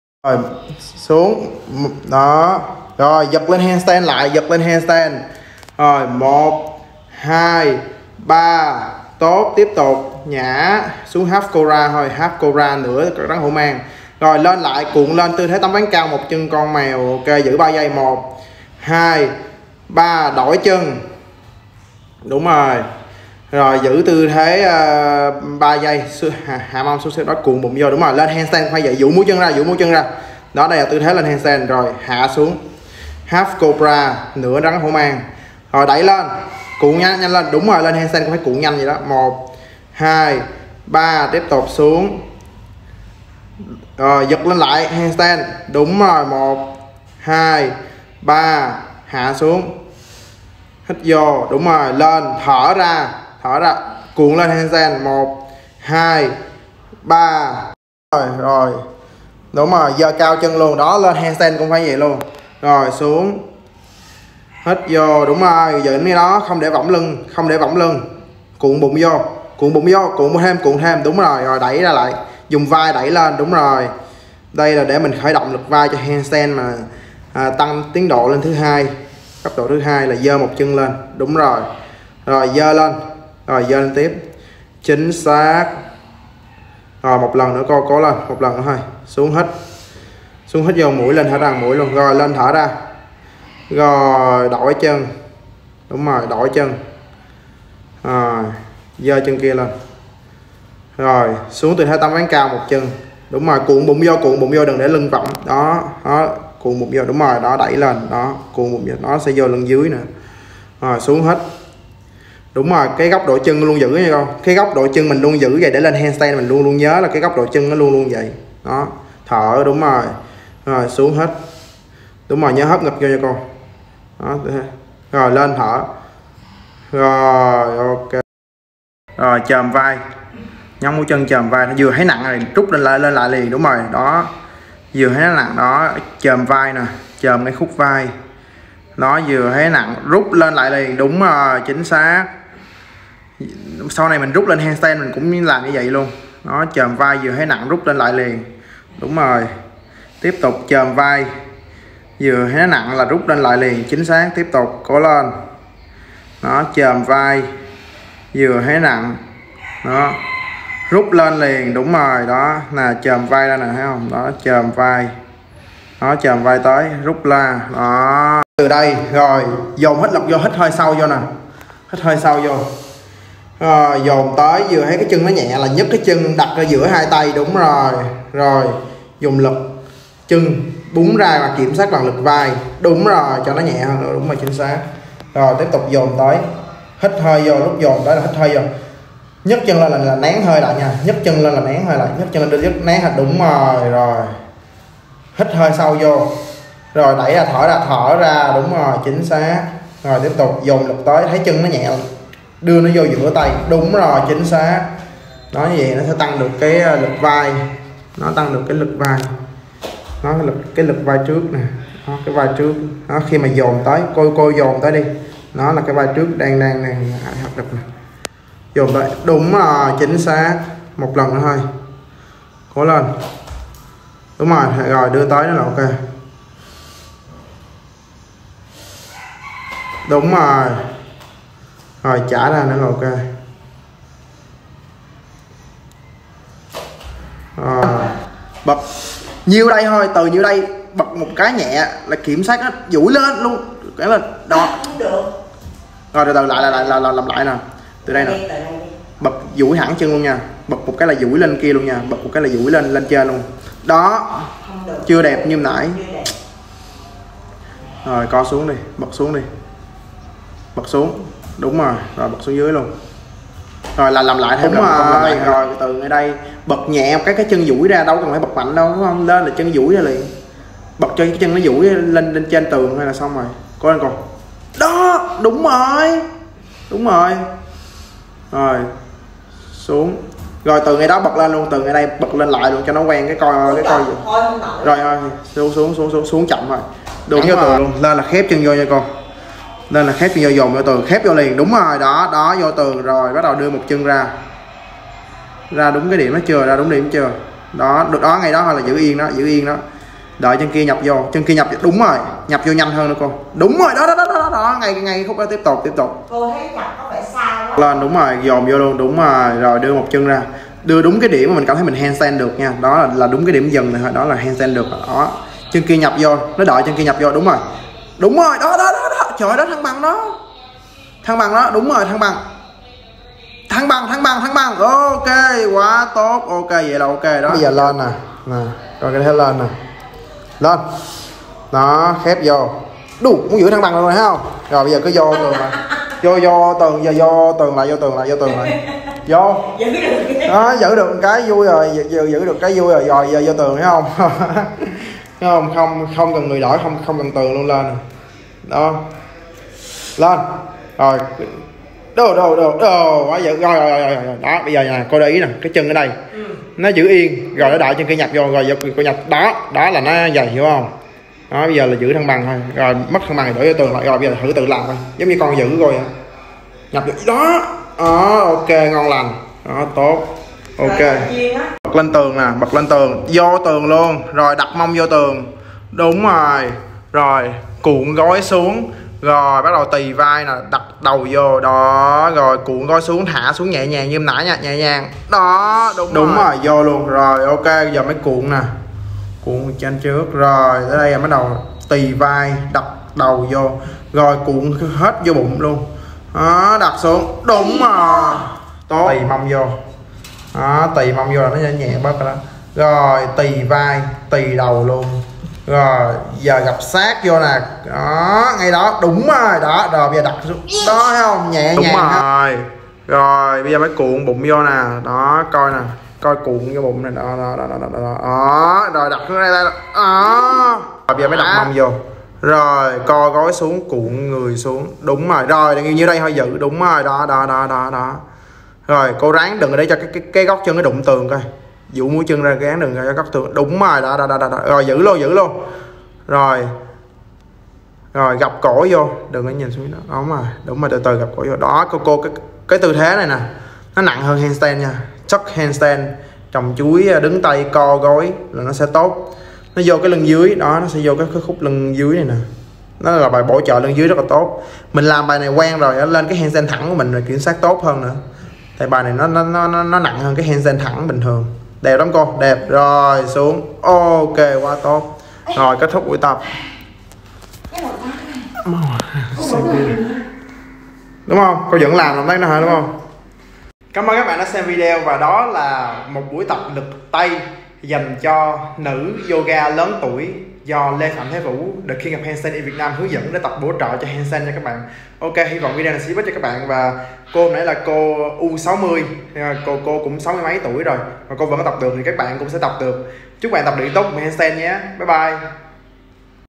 Hướng Dẫn Tập Lực Tay Vai HANDSTAND Cho Nữ Yoga Cao Tuổi Trồng Chuối Đứng Tay | Lê Phạm Thế Vũ
Thầy Giáo Thăng Bằng Tay Yoga Việt Nam - Thế Vũ 🇻🇳

Calisthenics & Street Workout Athlete from Vietnam - Le Pham The Vu  - 25 years old 🇻🇳